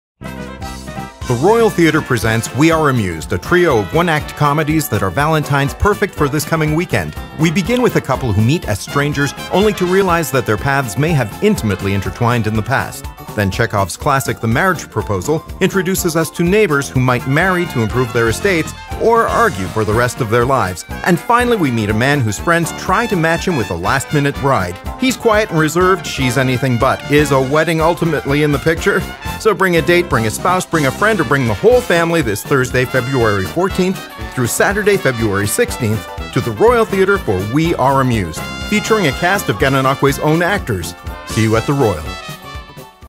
"We Are Amused" 3 One-Act Comedies!
Perfect for the Valentine's Weekend, the Royal Theatre Thousand Islands presents three engaging one-act romantic comedies!